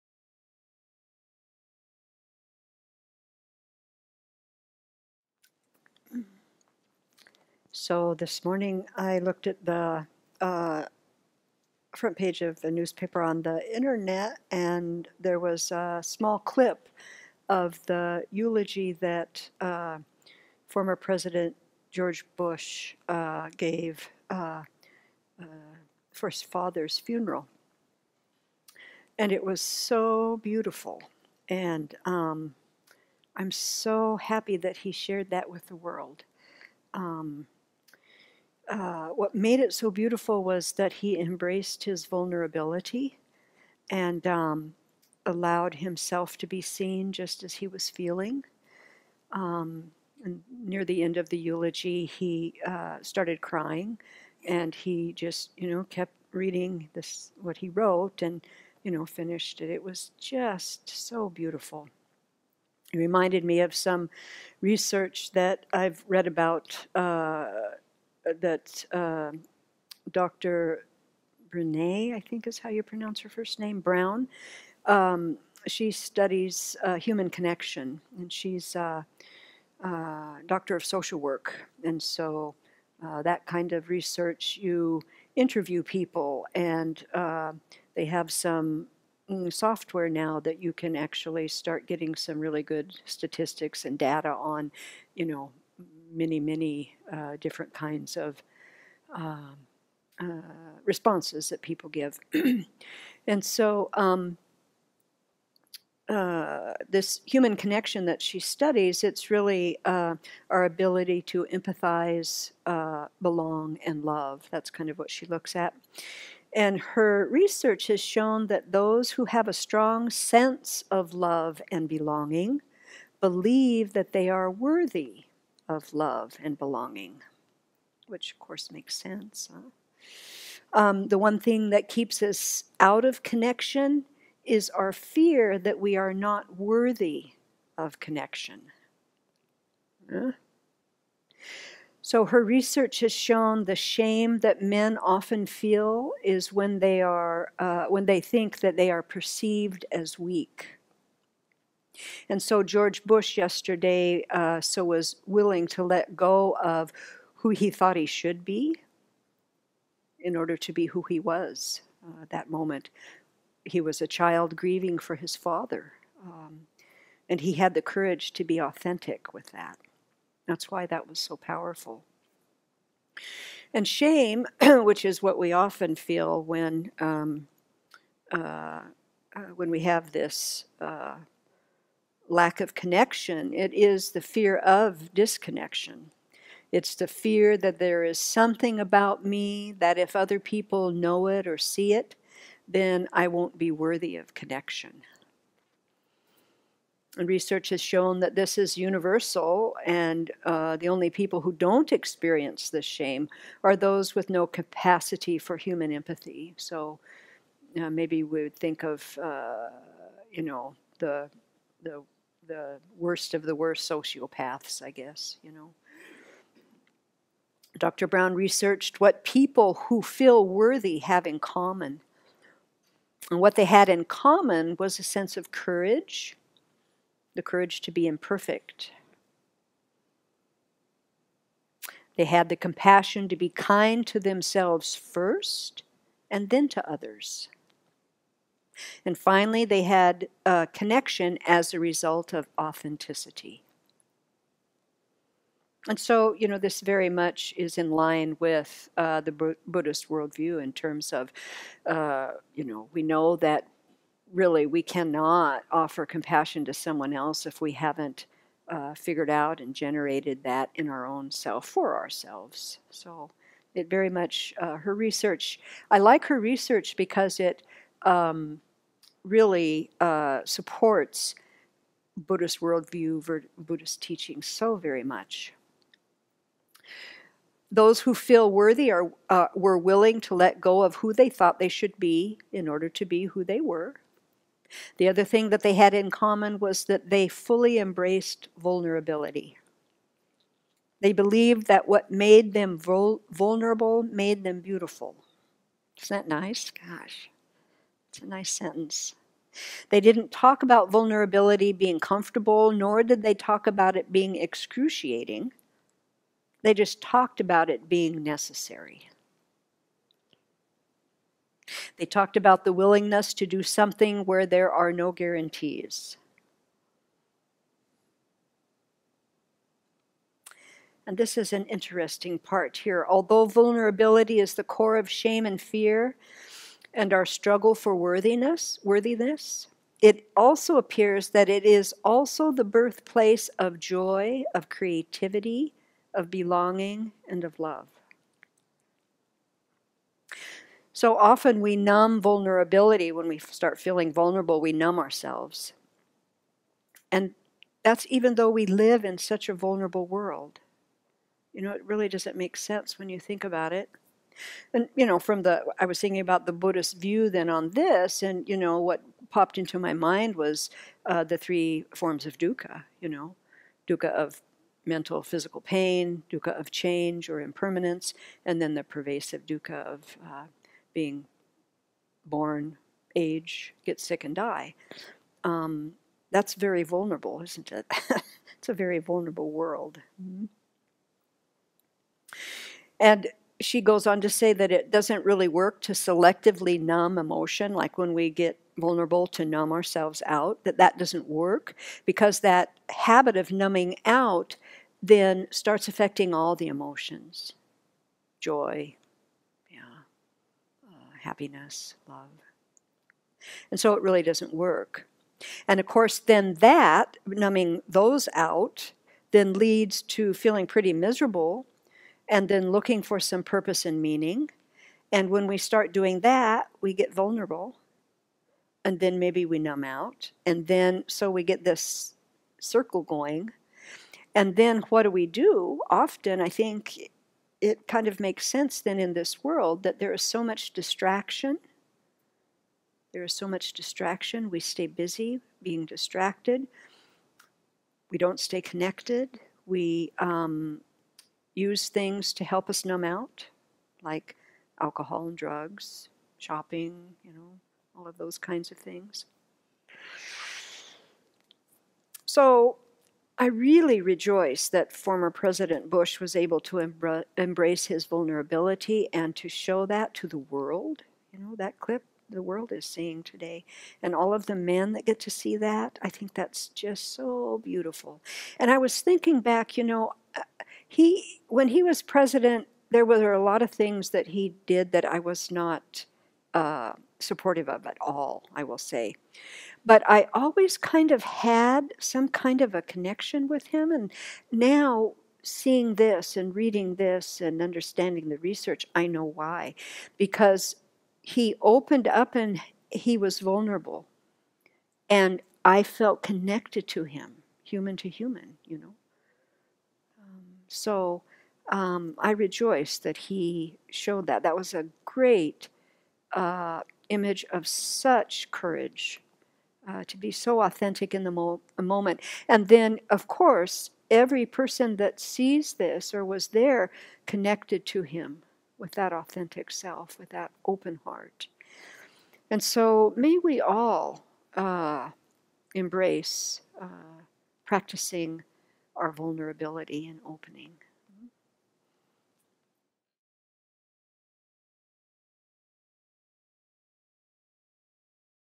12-06-18 Human Connection - BBCorner
Venerable Thubten Jigme speaks of former President George W. Bush’s eulogy to his father and the vulnerability that he willingly showed to the world, which she connects to the research of Dr. Brené Brown.